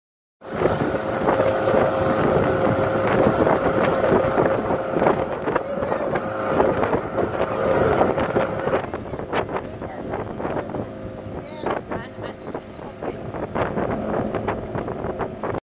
Boat trip in Oualidia
Boating is one of the best ways to discover Oualidia's lagoon and river estuary.